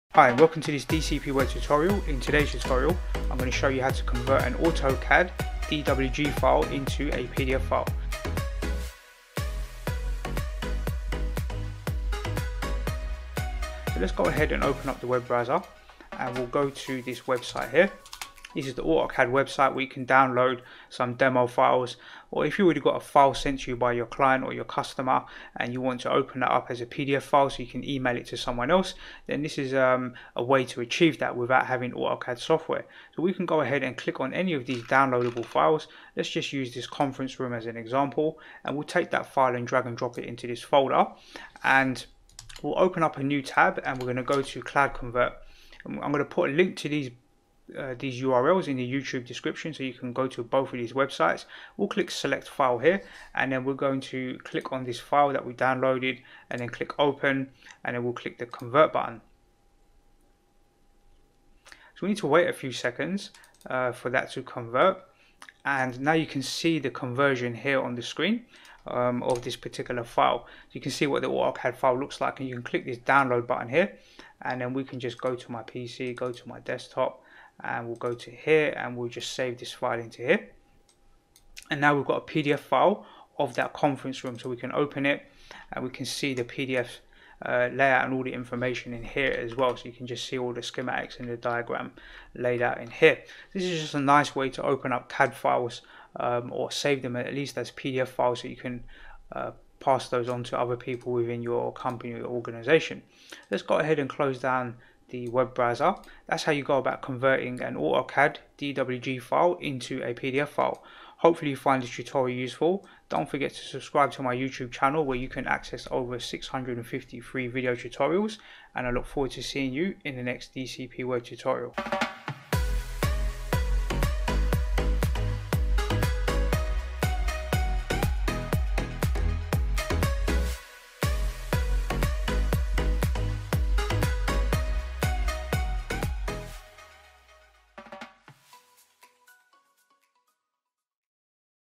How to Convert AutoCAD DWG file to PDF
In this video tutorial, I will show you how to convert AutoCAD DWG file to PDF.

Need a professional website for your business? - Simply contact DCP Web Designers for a free quote today

Related Keywords for this video tutorial